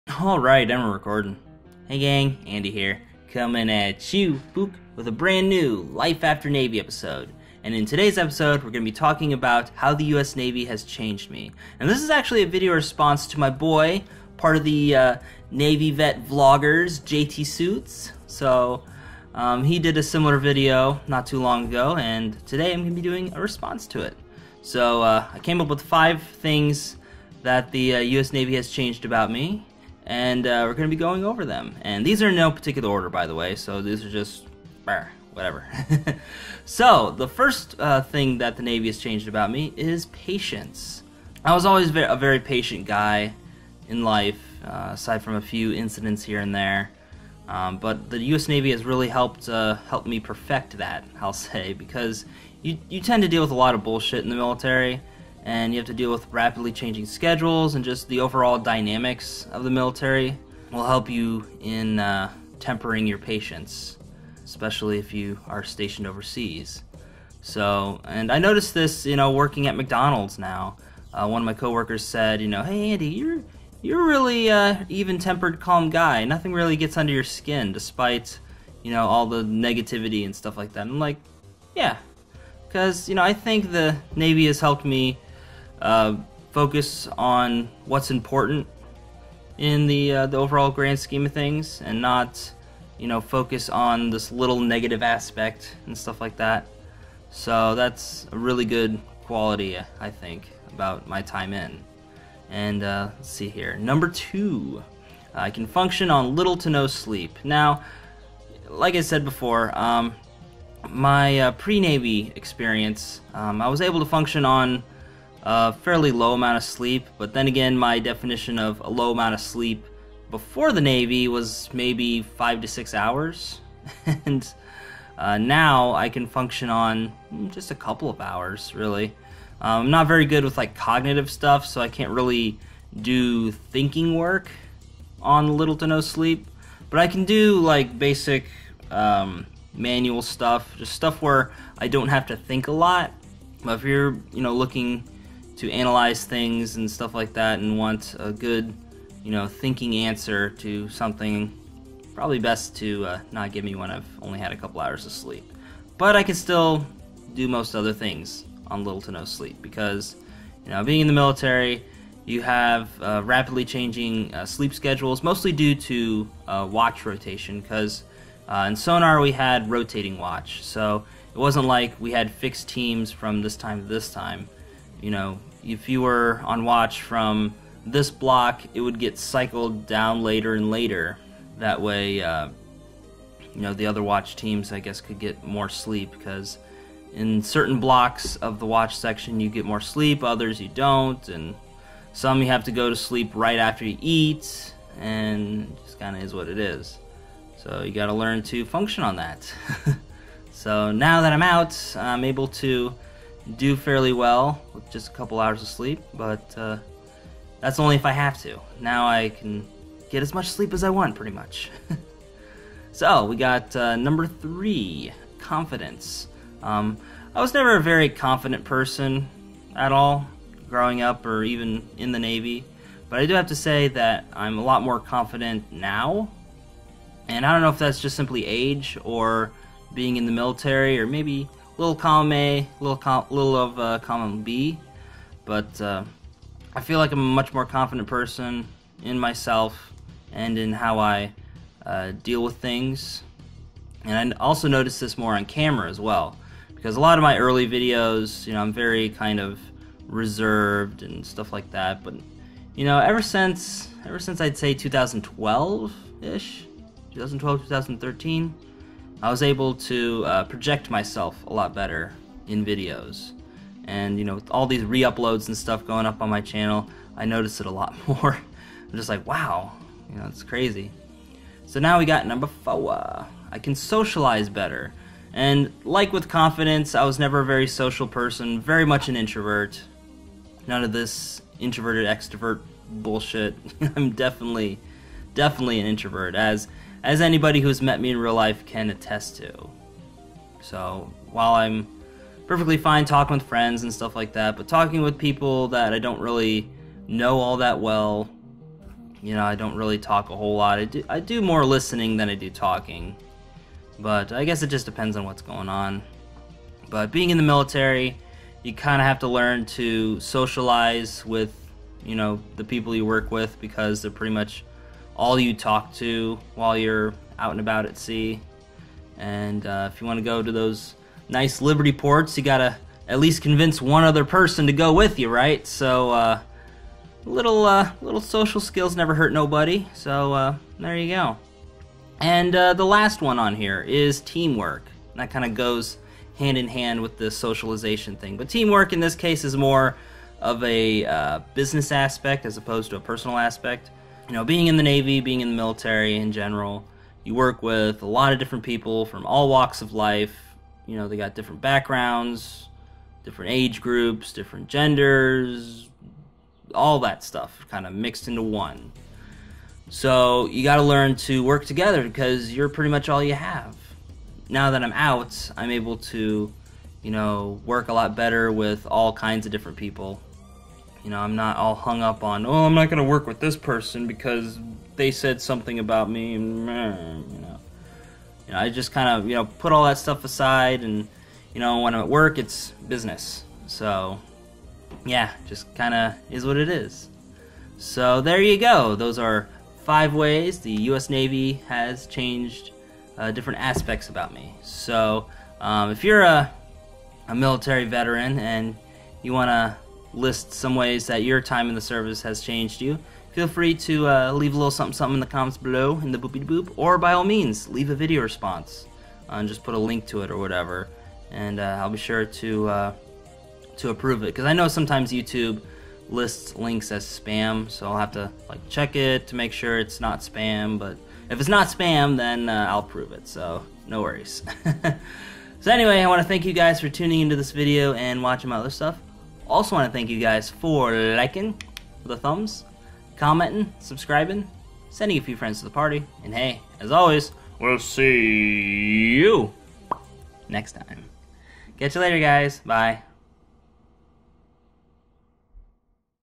How The US Navy Has Changed Me - Life After Navy 07
I talk about how the US Navy has changed me.

TheAndySan

TkyoSam

Radley

**Tech Notes**

*Camera Used*

*Mic Used*

ZOOM H2N

*Programs Used*

Audacity 2.1.1

*Music Credits*

BGM Artist: Protricity